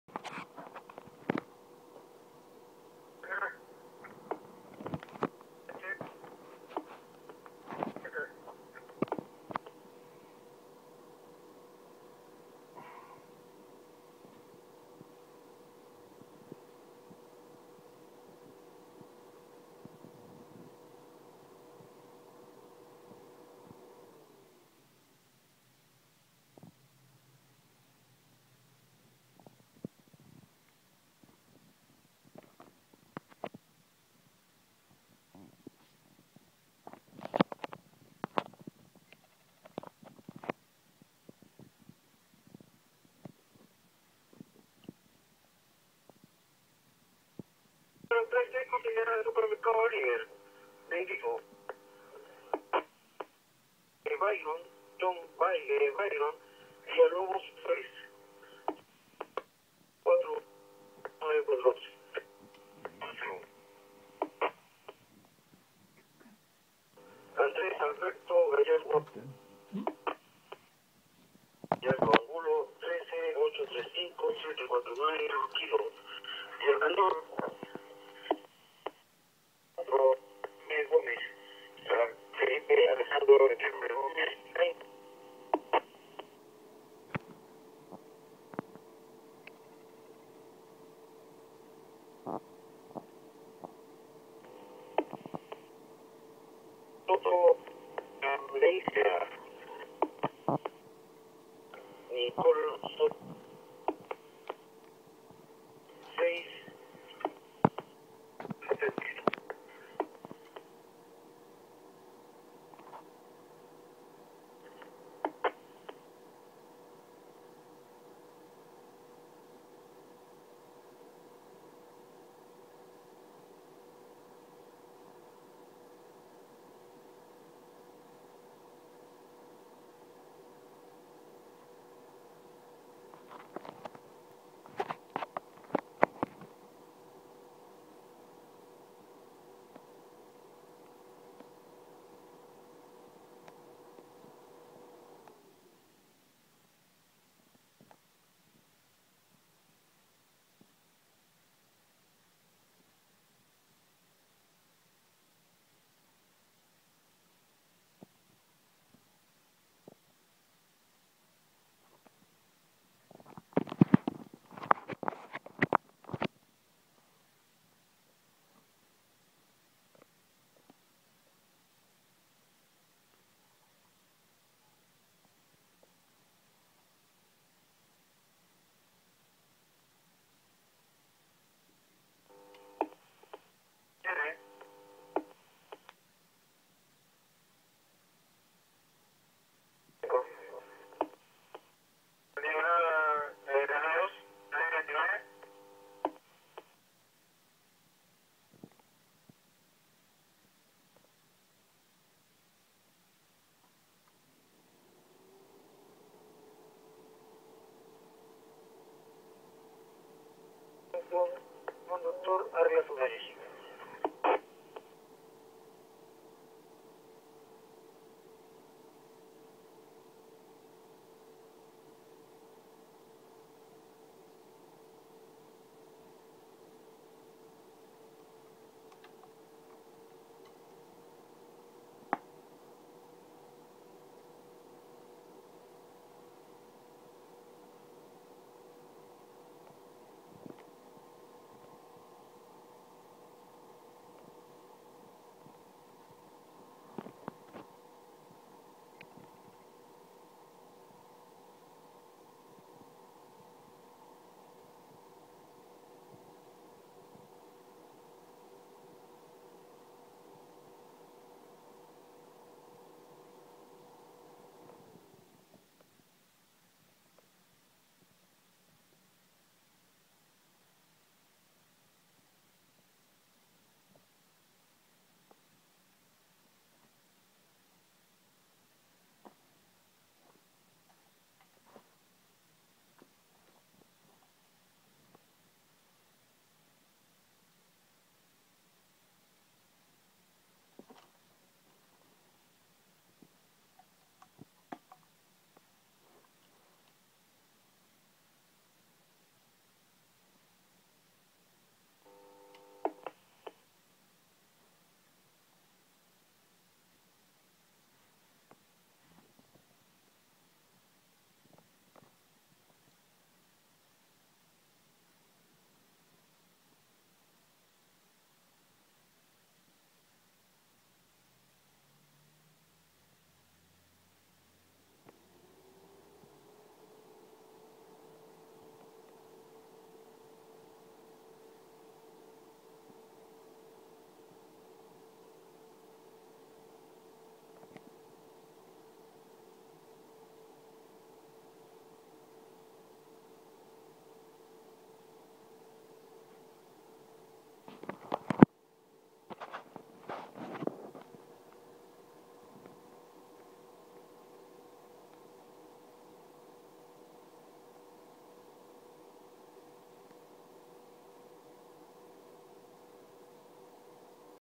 Frecuencia Carabineros Cordillera 3 Apco25 Encriptado Recibido en Motorola DGM6100
Interesante lo que Apareció la Frecuencia de Carabineros de Cordillera 3 interesante por decirlo menos esto del Trk 800mhz APCO25 Encriptado apareció esto. Recuerdos de mi TH-77A hace millones de anos atrás cuando todo era análogo